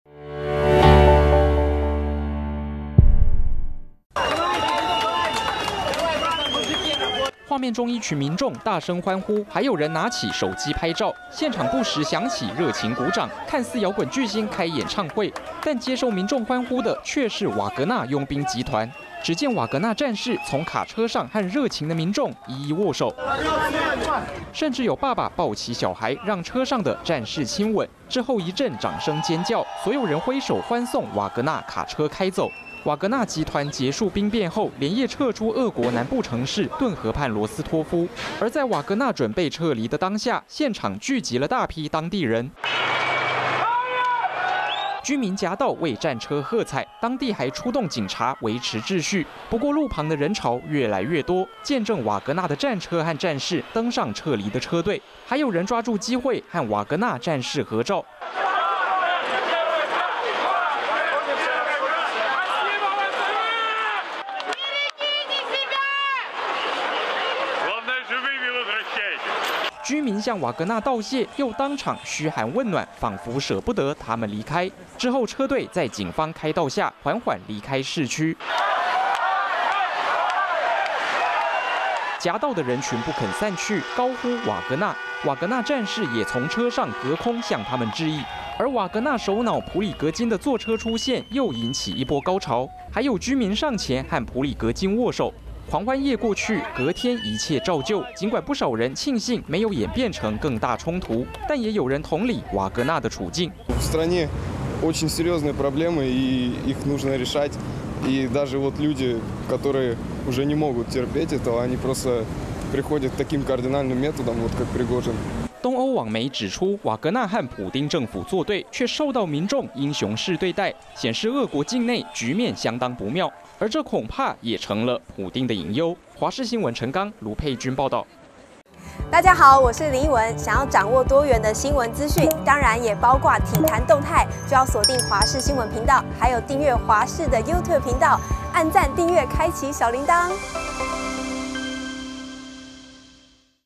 瓦格納結束兵變撤離 羅斯托夫居民夾道歡送｜華視新聞 20230626
全球關注的俄國瓦格納兵變，儘管短短24小時就結束，而被迫退讓的俄國總統普丁，也被認為相當沒面子，不過讓他更沒面子的是，瓦格納傭兵準備撤離占領區時，竟然受到當地居民的英雄式歡送，而且居民甚至搶著和傭兵戰士握手合照，在他們離開前大聲加油打氣，又是噓寒問暖的，有東歐網路媒體指出，這樣的景象顯示，俄國境內的局勢可能相當不妙。